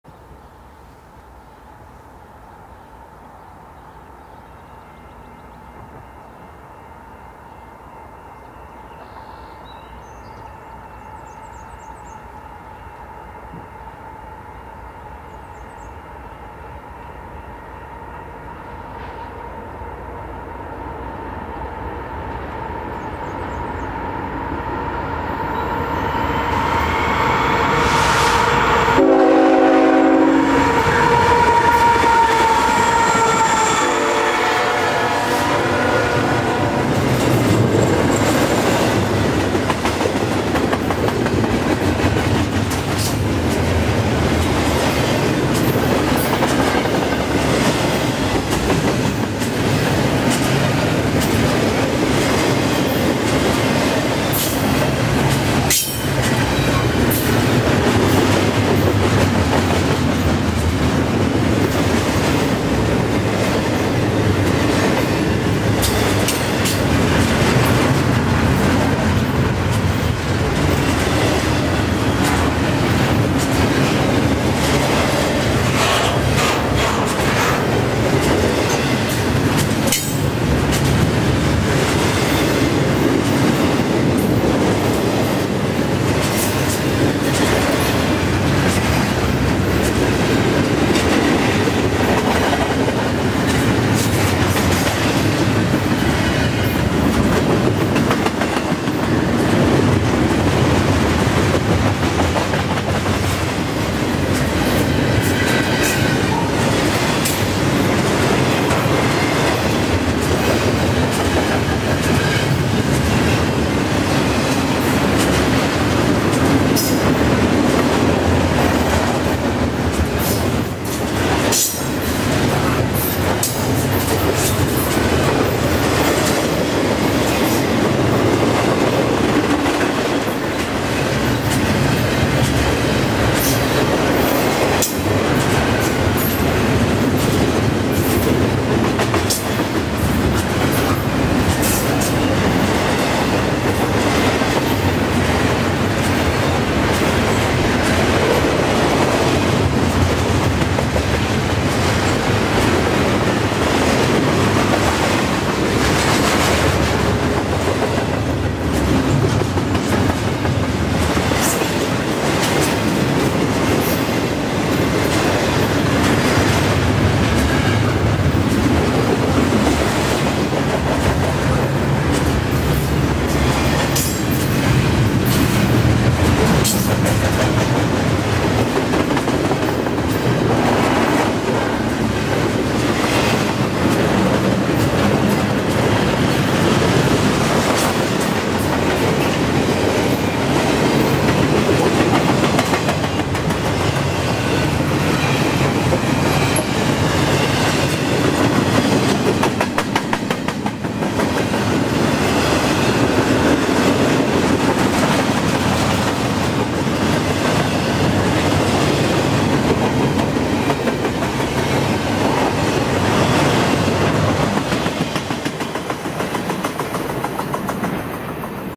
CN Train 149 Westbound May 18, 2023
Engines: CN 2842, CN 3032
Cars: 163 (wells)
Speed: 39 mph at detector 124
Location: Brockville VIA station, ON, Canada
Temperature: 9C
Origin: Montreal Intermodal QC
Destination: Chicago Intermodal IL